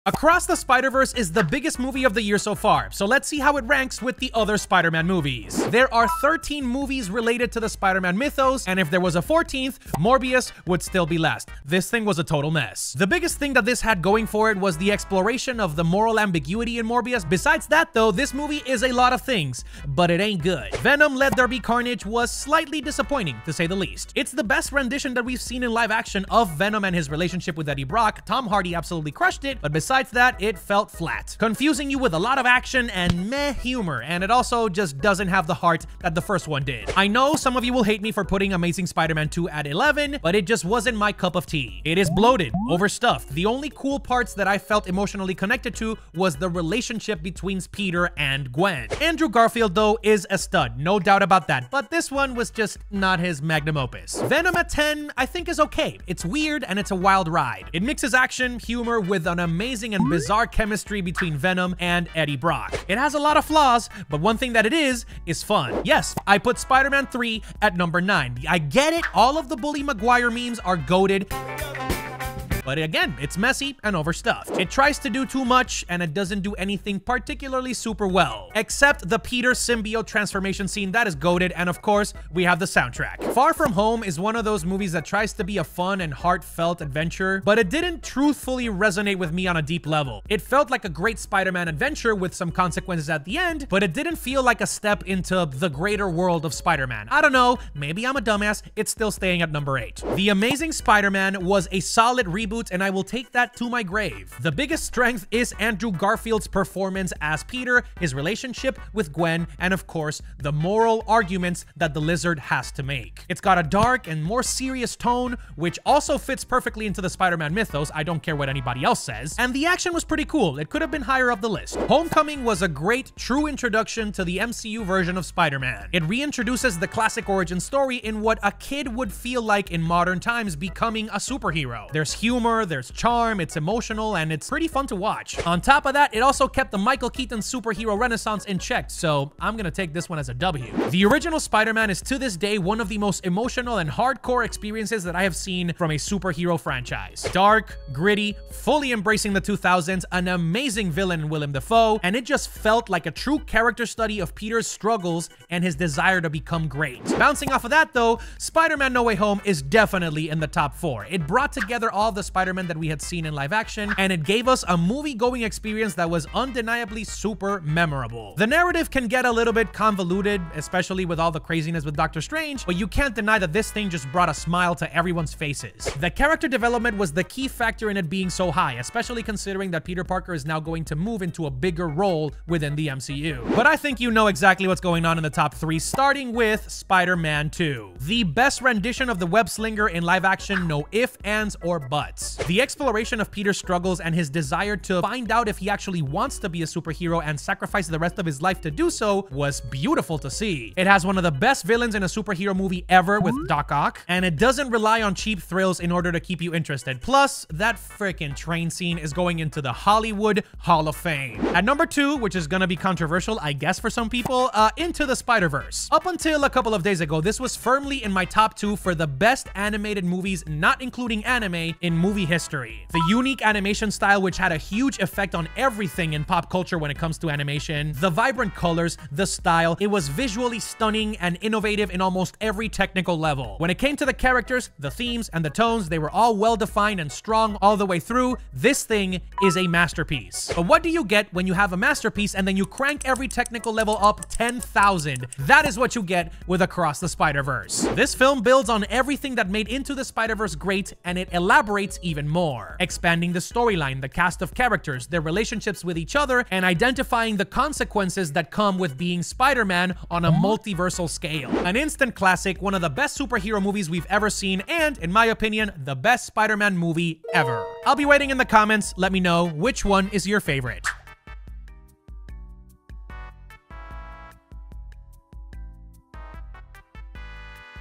ALL Spider-Man Movies Ranked
Across The Spider-Verse is the biggest film of the year so far, so let's see how it ranks with the other Spider-Man movies. Do you agree with my rankings of The Spider-Man movies?

Was Shazam Fury of the Gods That Bad?